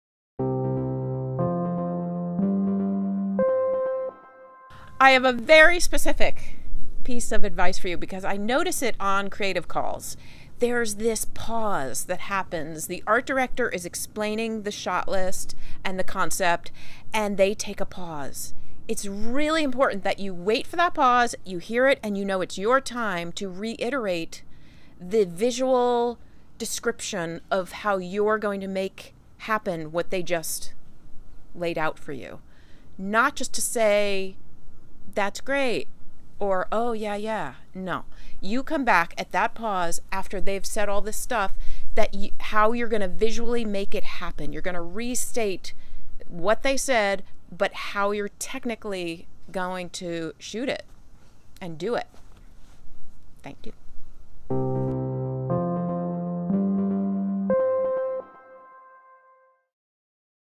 AskSternRep on Creative Calls 'Make or Break' Moment
Commercial Photographer's Rep Andrea Stern of SternRep + AskSternRep discusses creative calls and a tip to use during them.